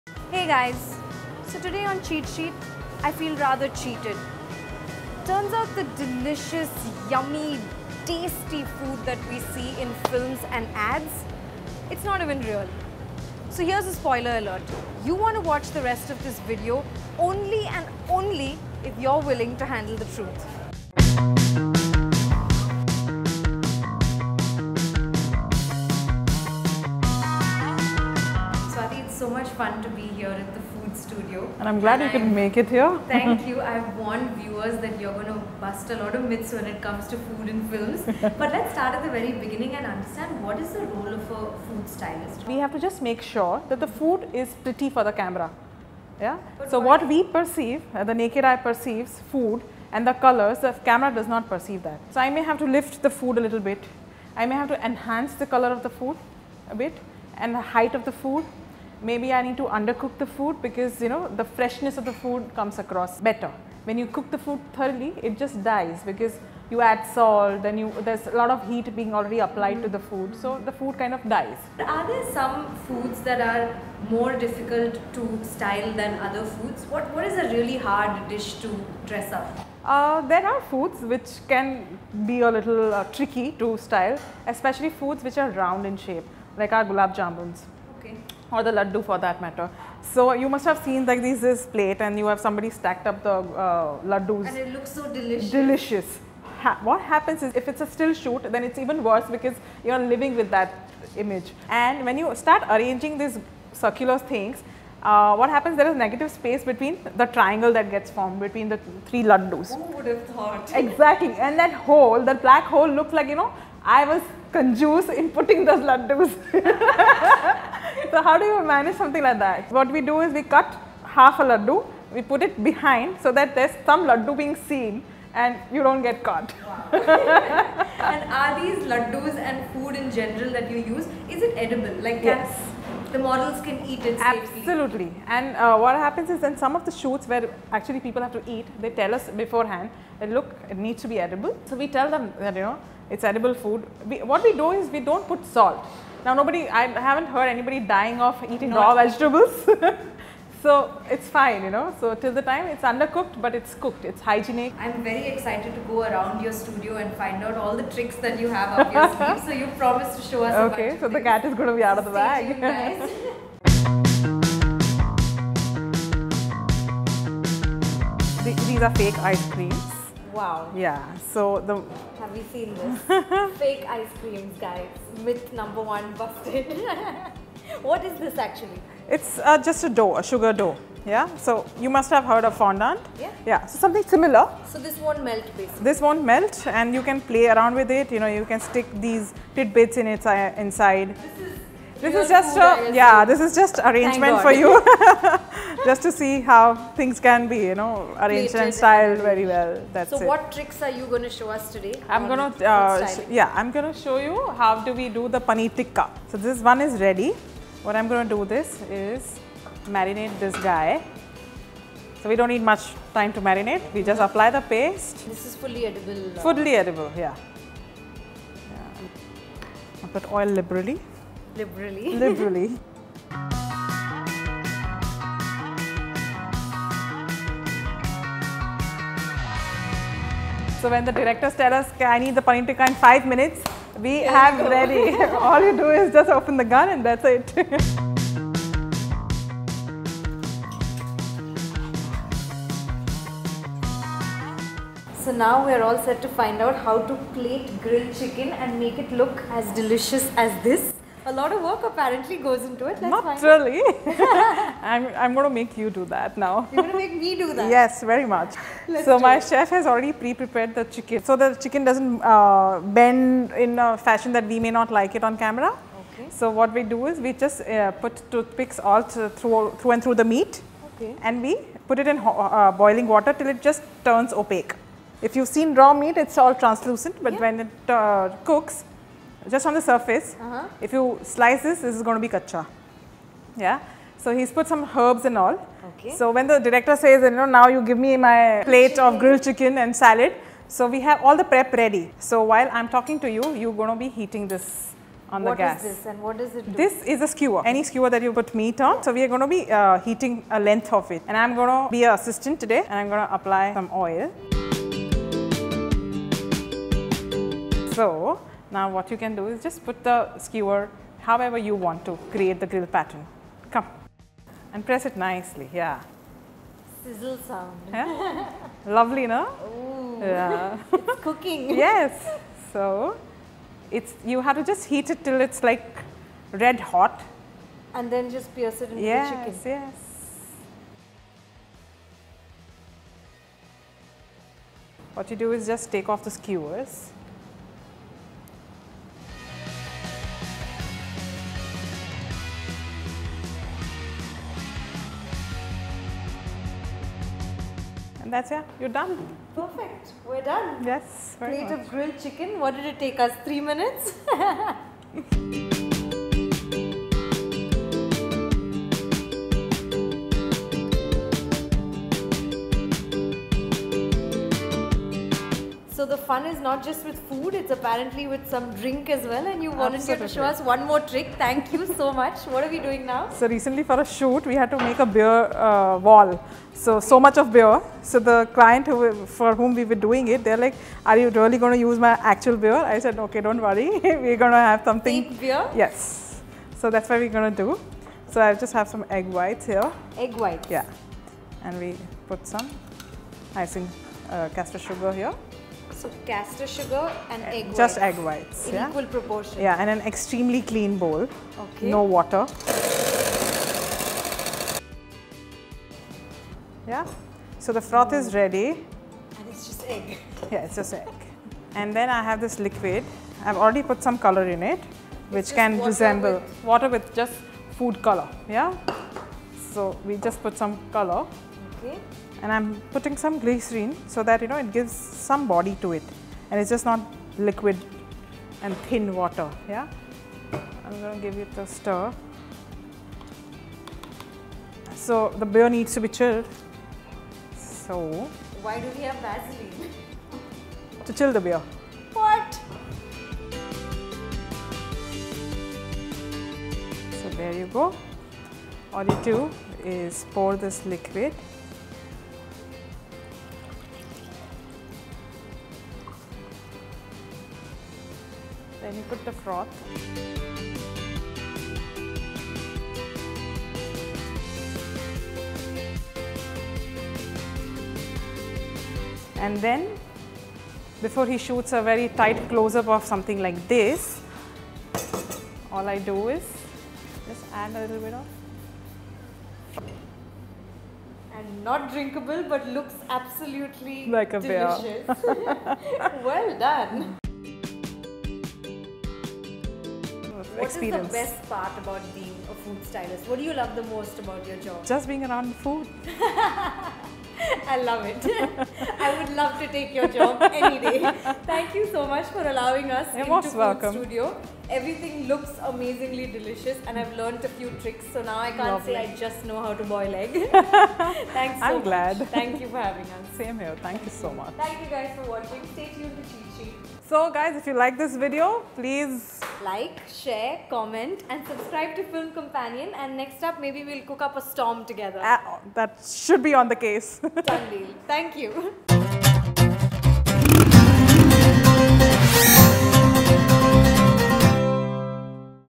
How To Style Food For Films | Cheat Sheet | Sneha Menon Desai
On Cheat Sheet, we meet Swati Desai of the Food Studio to understand some of the most interesting food styling hacks.

From how to make fake beer and grilled chicken for the cameras, we show you how to make food look like a million bucks.